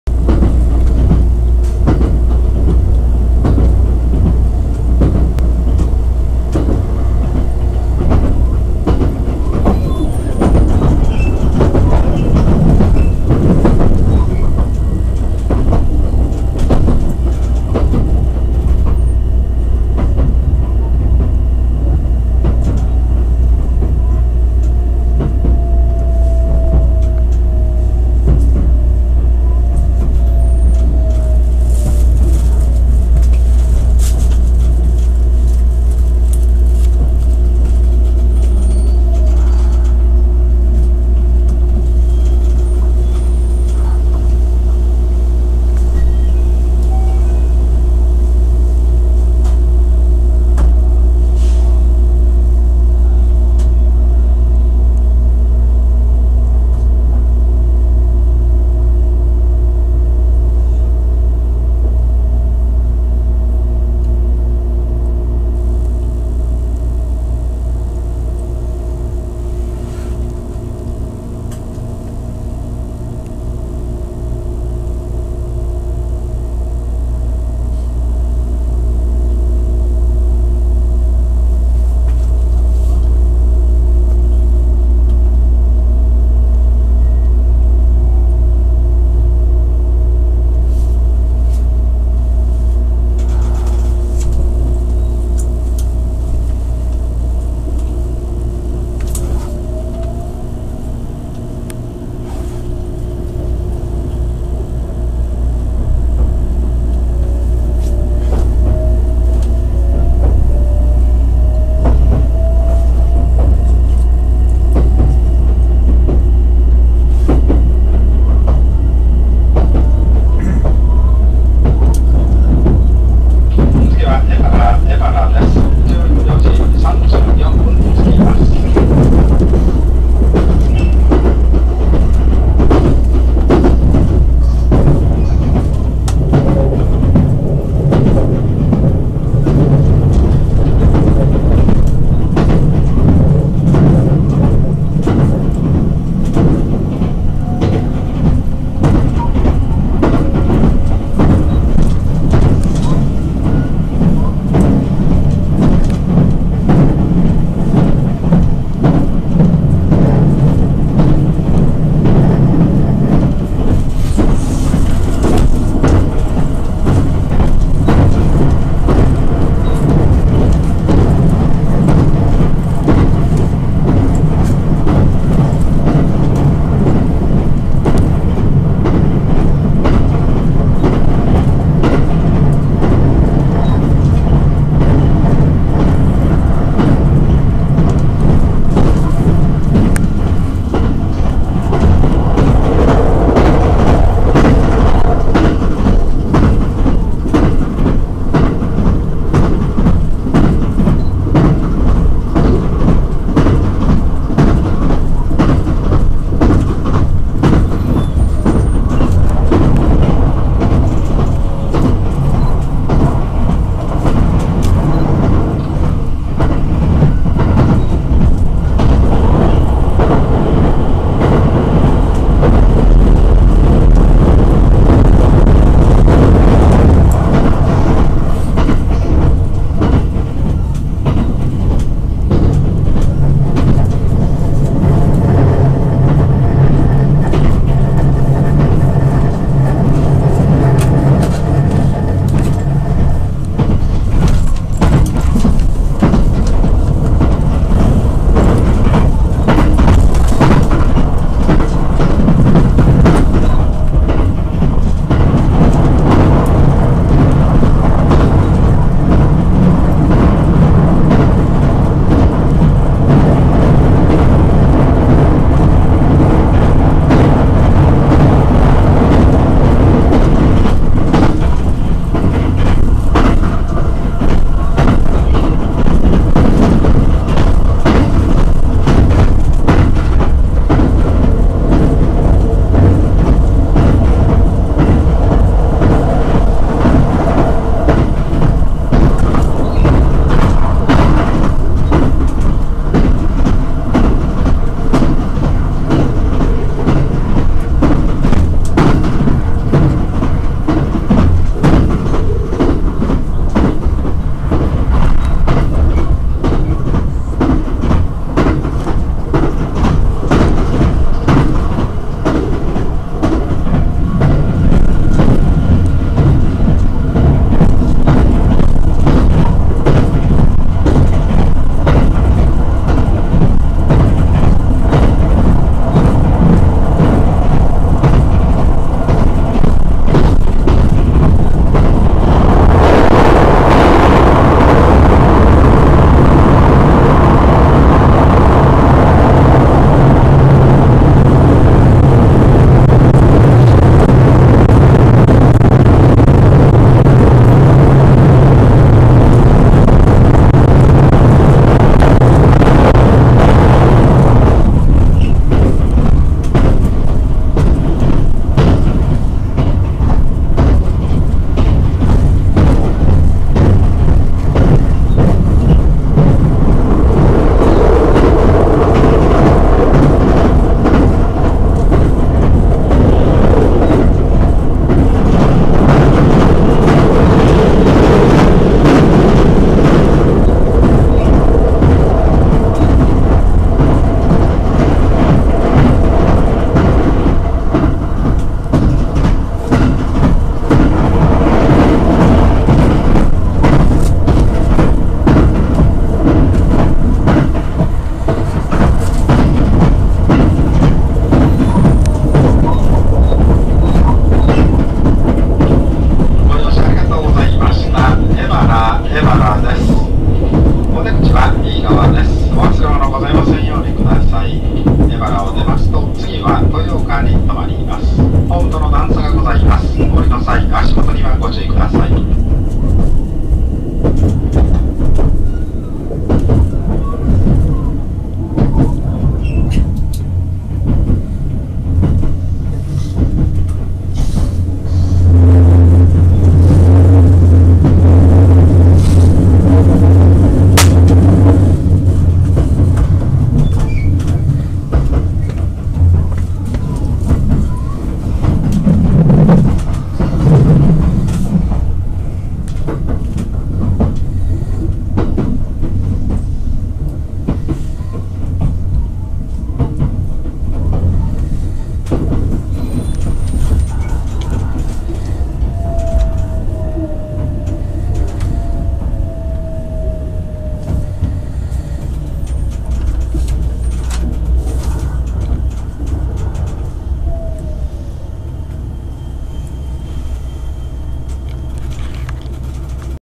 こうのとり9号八鹿→江原(2012.6.10).MP4
新型車両の特急こうのとり9号に乗って城崎温泉へ行きました。　やや横揺れは感じましたがスピード感豊かで乗り心地は最高でした。　枕木をコンクリにしていただければ更にスムースな走りができるのではないでしょうか。